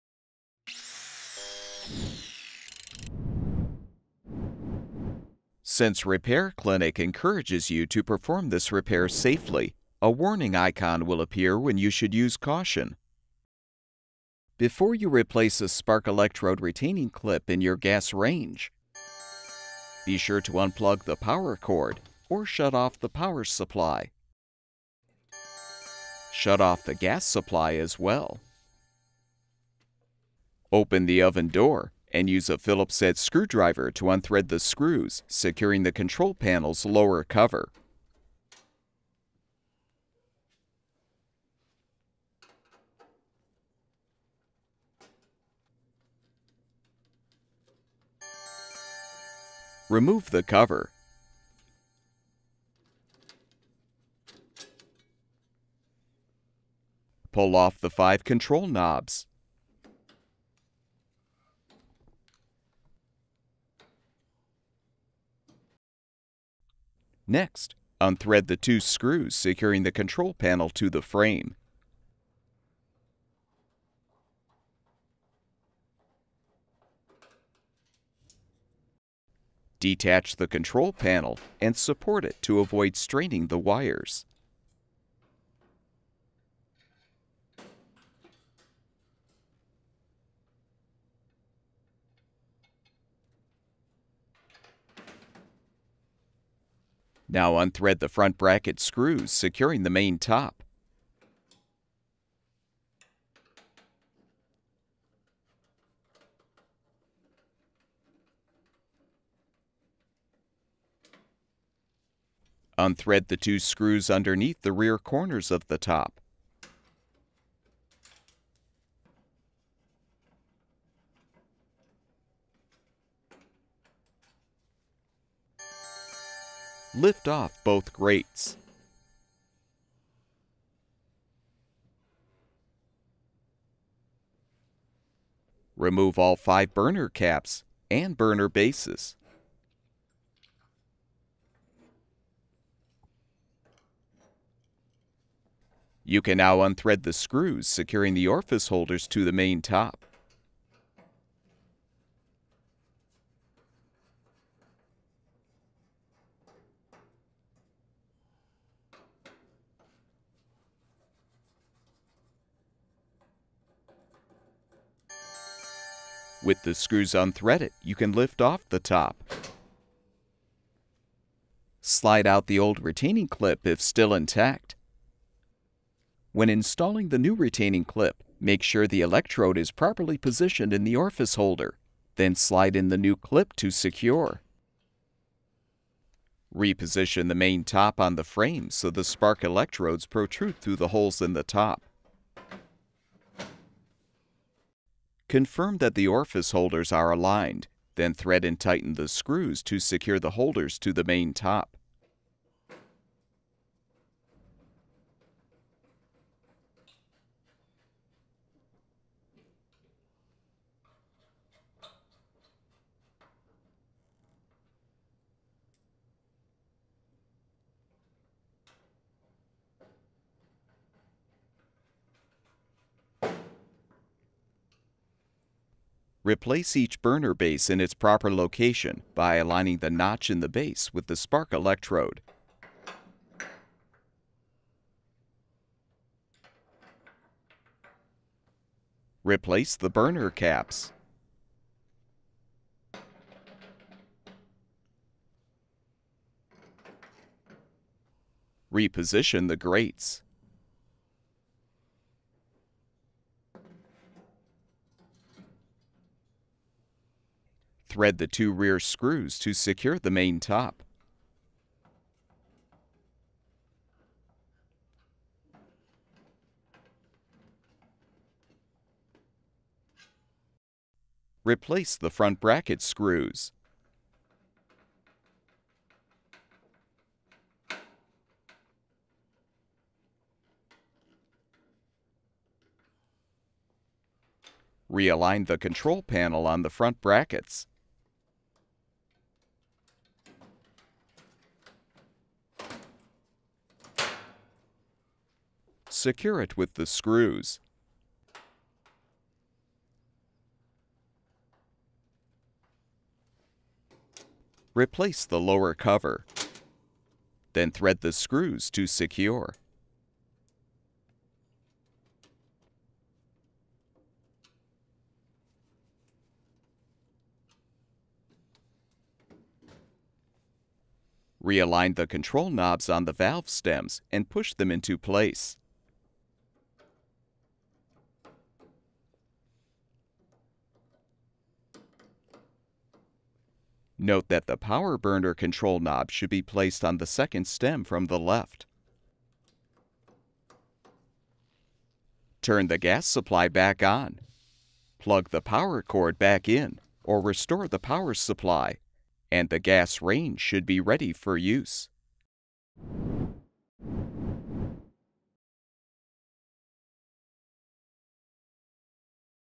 KitchenAid Convection Gas Range Spark Electrode Clip Replacement W11190173
This video provides step-by-step repair instructions for replacing the spark electrode clip on a Kitchenaid Gas Convection Range (model KSGB900ESS01). The most common reason for replacing the electrode clip is if the electrode is not secured to the orifice.

This electrode replacement video is applicable to the following brands:
Whirlpool, Kitchenaid